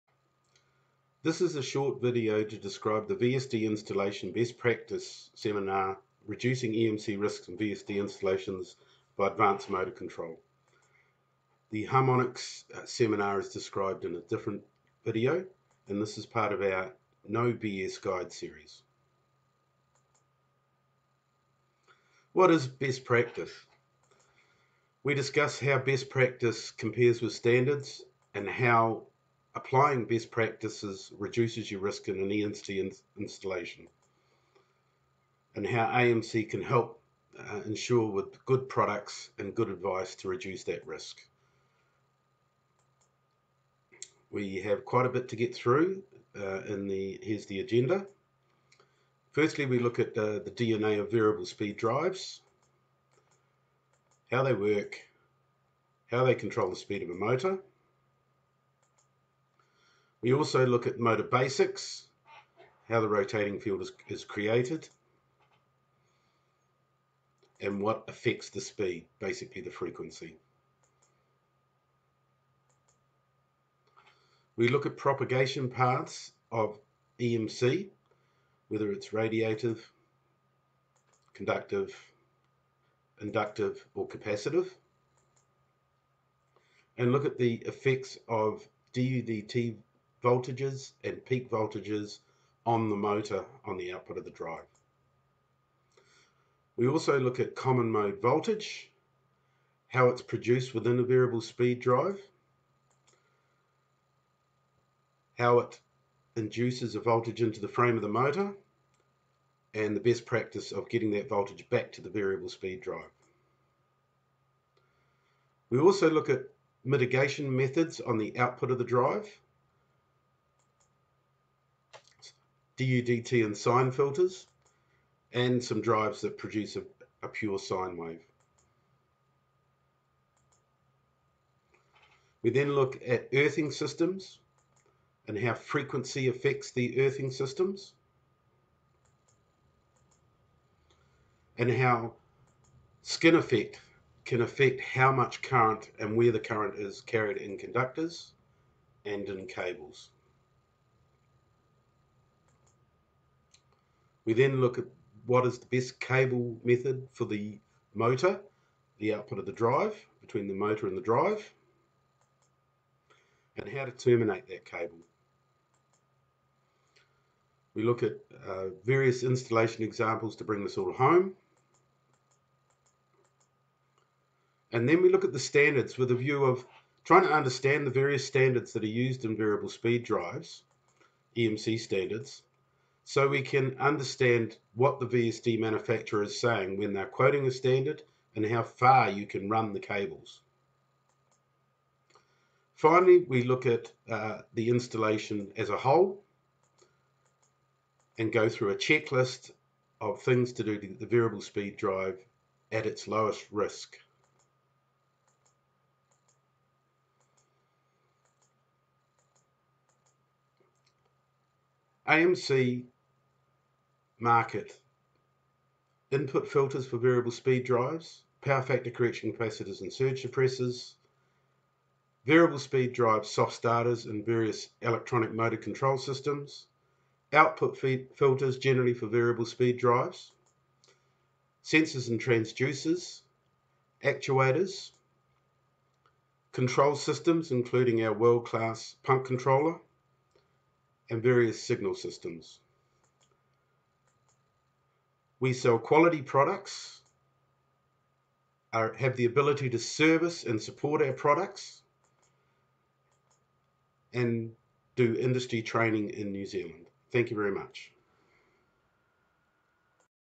EMC & VSD Installation Best Practice 2020
This video describes the content of the EMC & VSD installation best practice seminar held in New Zealand